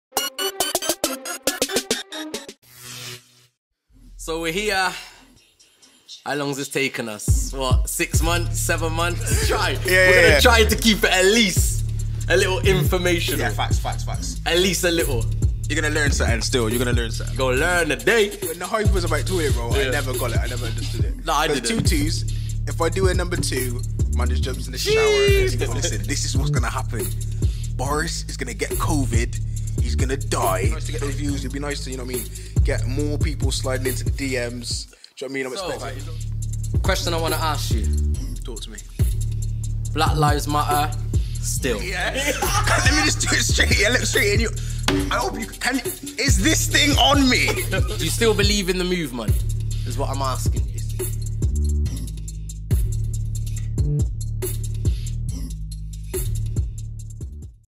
Introducing The Broken Generation - Episode 1 | Promo Trailer
Are we living in a Broken Generation!?

Welcome to The Broken Generation Podcast Show |
Open, honest and real discussions around everyday issues that effect society. Hosted By Last Name Henry & Danger

Join the discussion on Instagram  @Broken_Generation_Podcast

Filmed and edited by Sophisticated Rebel Media